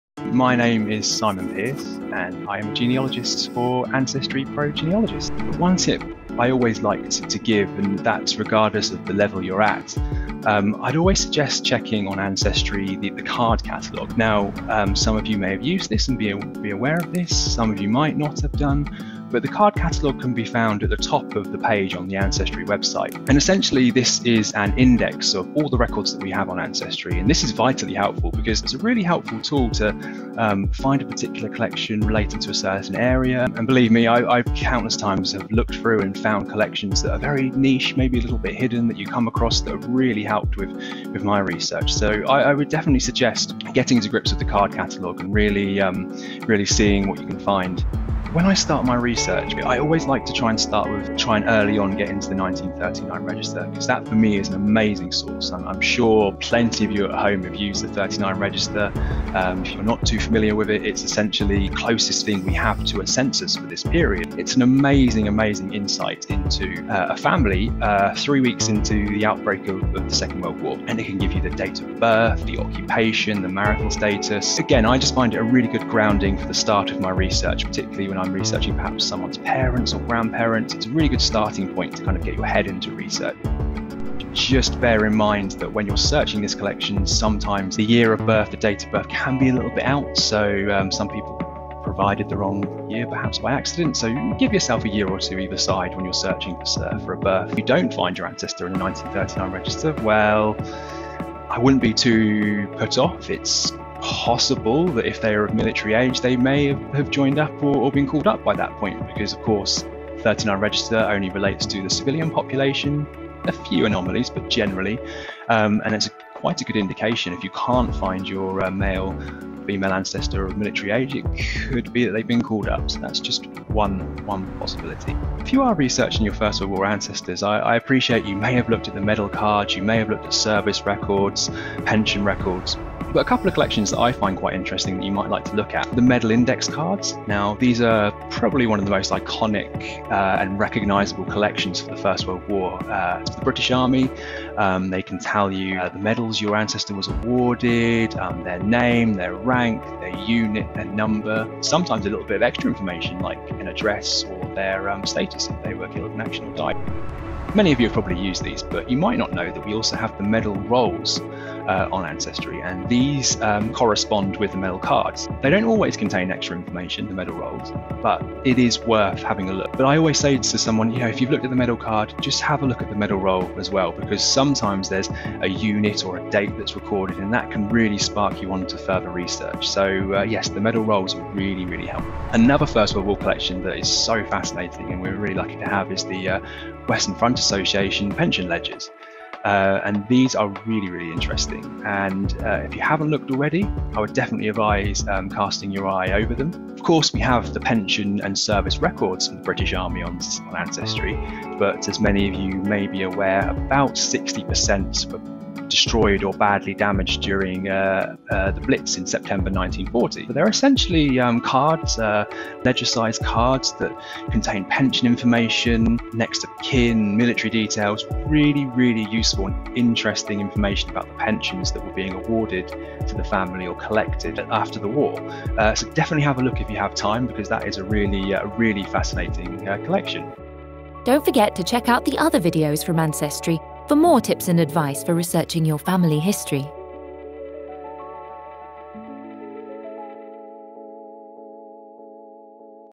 Tips For Starting UK Family History Research w. Simon Pearce | Ancestry UK
Learn how to start your UK Family History Research using these tips from Ancestry expert, Simon Pearce.